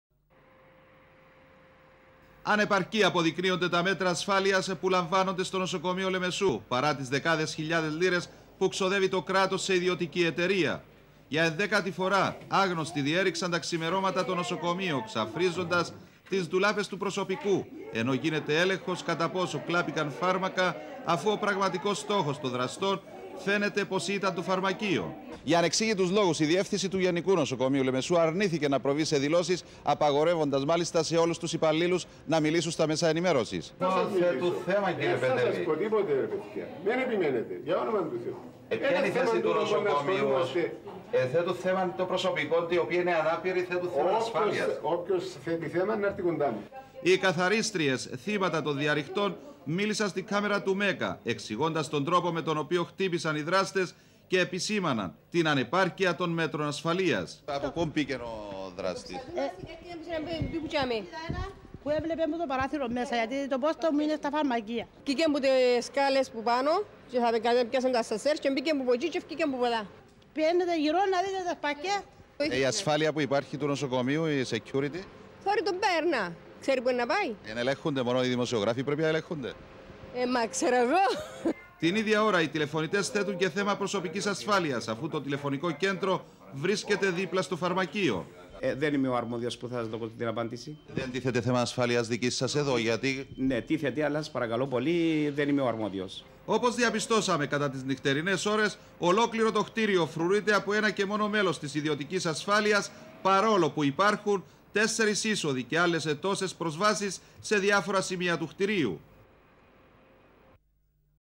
Νοέμβριος 2000 Διάρρηξη νοσοκομείου Λεμεσού, διευθυντής Ανδρέας Πέτεβης
Ρεπορτάζ: Πάμπος Μιτίδης
Κάμερα: Κλείτος Ερωτοκρίτου
Μοντάζ: Ανδρέας Μιχαηλίδης
Αρχείο: Videoscope studio